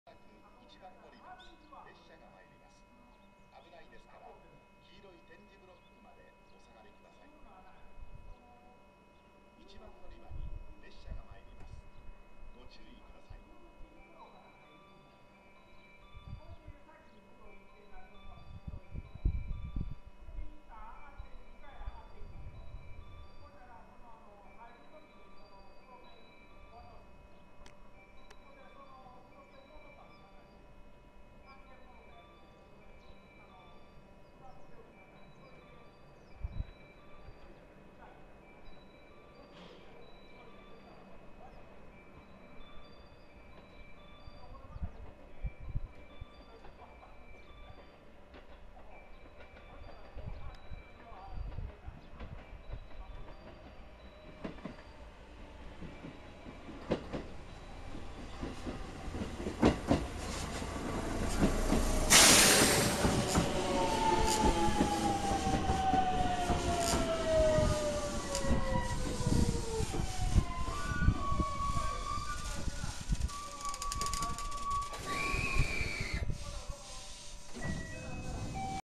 223系@稲原駅
平成24年2月12日、JR稲原駅の1番のりばに入ってくる223系。御坊発紀伊田辺行の普通電車。列車番号2353M。行き先表示が御坊になっているのはご愛嬌？